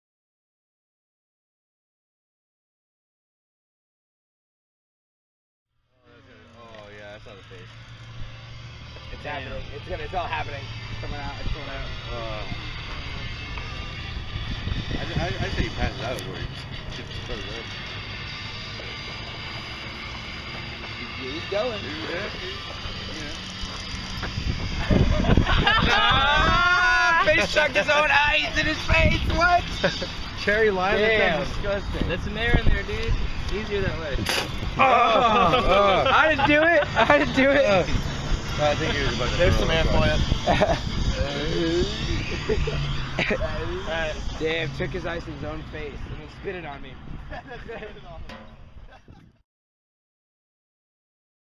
ICED Erich Fraustro
Boi Miller ICED Erich with a hot Smirnoff while we where at the Skate park in Arizona.
The temperature outside was 117!!
What would we do with out our FRIENDS!!